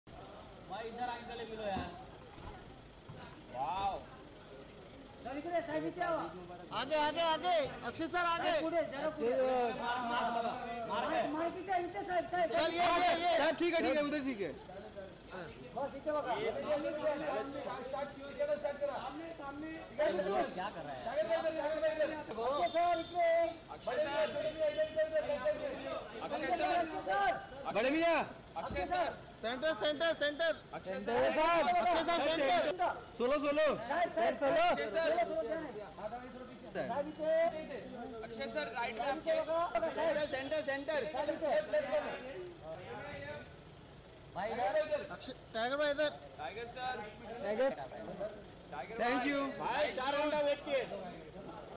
ଏକା ସାଙ୍ଗରେ ନଜର ଆସିଲେ ଟାଇଗର ଓ ଅକ୍ଷୟ କୁମାର ।
ନିଜ ଫ୍ୟାନ ମାନଙ୍କ ସହ ନଜର ଆସିଲେ ନୋରା ଫତେହି ଏବଂ କୁନାଲ ଖେମୁ ।